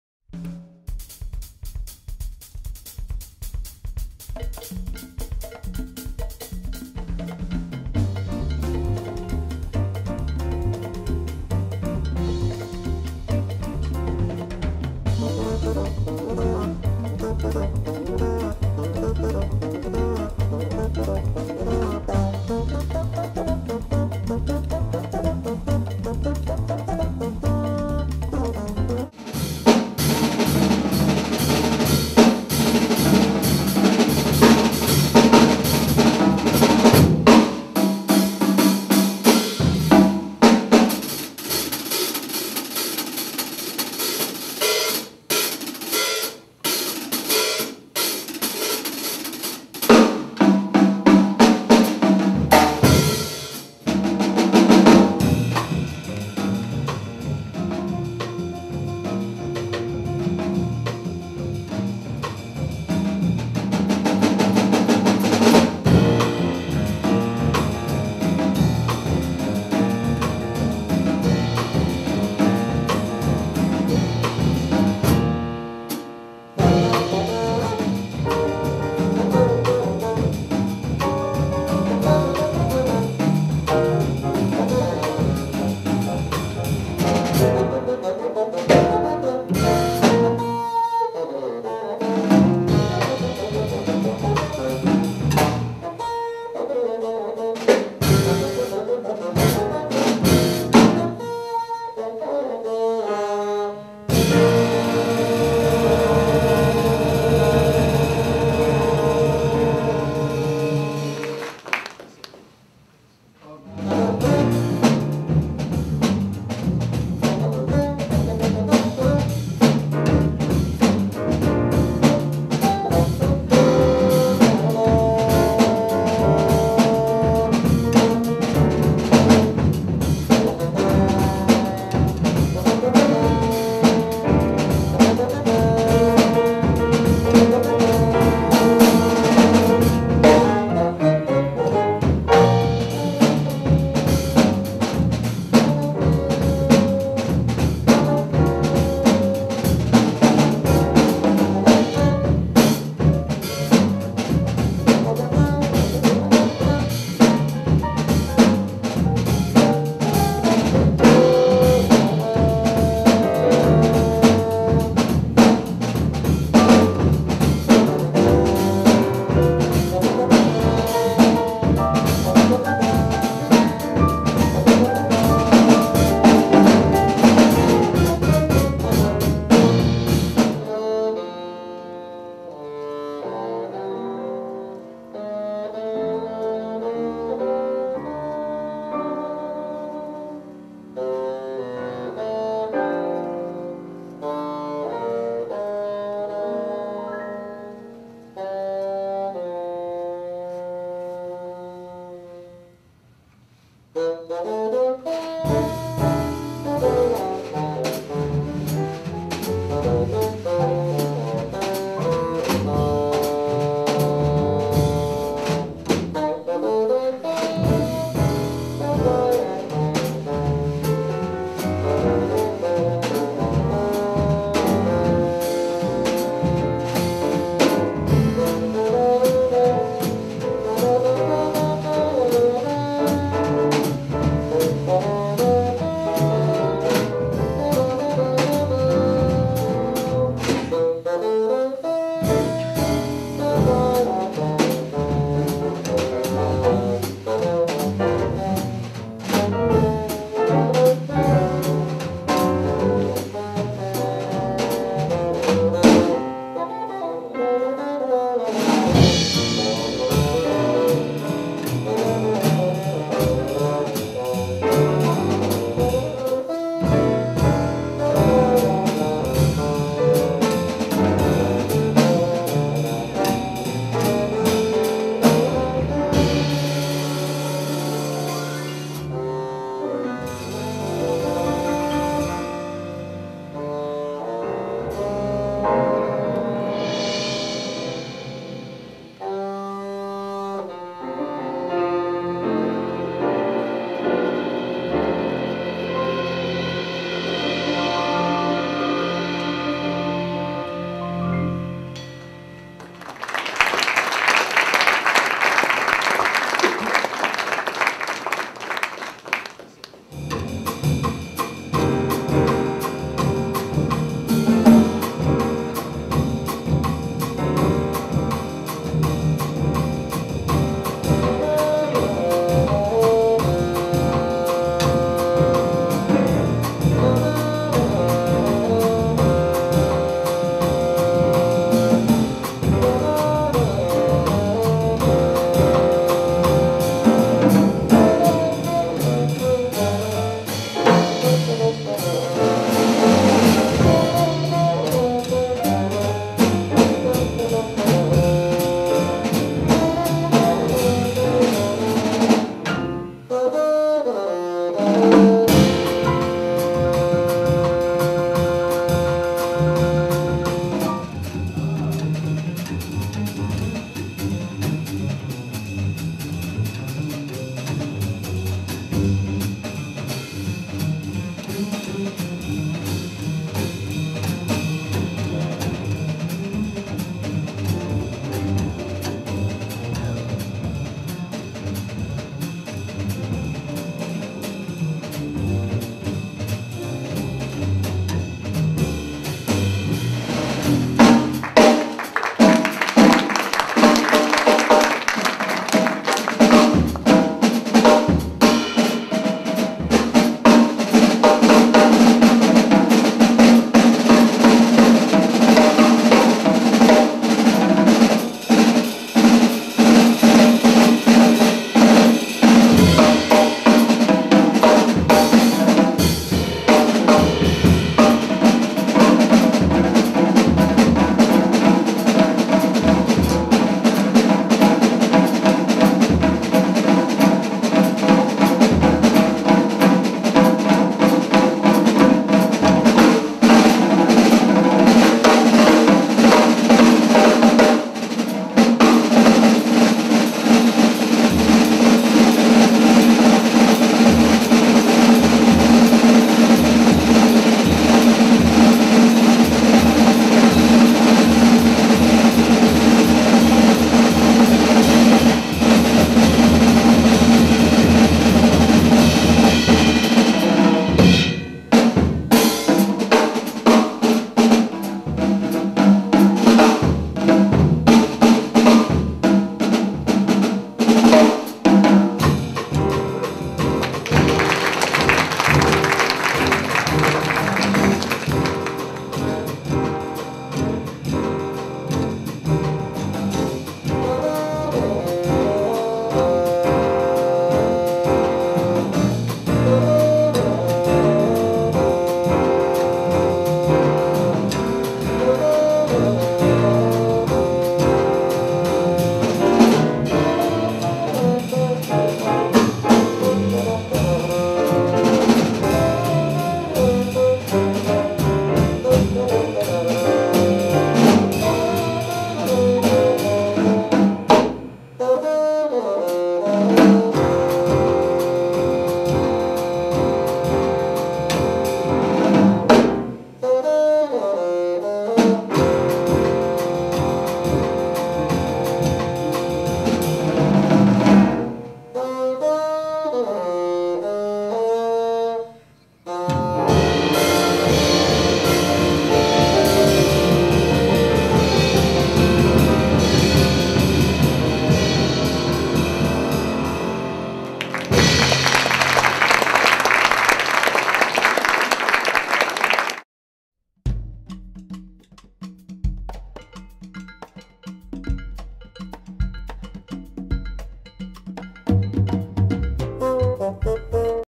Daniel Smith's Bassoon & Beyond at Jefferson Medical College, Philadelphia 2013
The Dean's Concert Series at Jefferson Medical College, now in it's third year, is a one-of-a-kind venue at an American medical school.- a setting where world-class musical performers come face-to-face with not only faculty but also medical students, and add a musical dimension to their lives. The message is clear-music has a role to play in healthcare and those delivering it.

This February, the renowned bassoon virtuoso Daniel Smith, with his acclaimed jazz quartet 'Bassoon and Beyond', filled our concert hall, with a capacity audience, to memorable and well received jazz riches. His performance showcased to Jefferson students and faculty the way a bassoon can be invoked to create a new sound in the jazz world. This is a striking example of musical 'cross-over', whereby a classical music instrument can be reincarnated as a jazz instrument.

The Dean of the medical school picked up on this point in his closing comments, noting that 'cross-over' is as relevant to creativity in the biomedical sciences as it is to musical creativity. This concert also represented Daniel Smith's return to the concert stage after an interlude in his life during which he was grappling with family health issues, making this particular performance, in front of a medical audience, all the more meaningful.        -- Mark Tykocinski, Dean Jefferson Medical College, Philadelphia - February 19, 2013.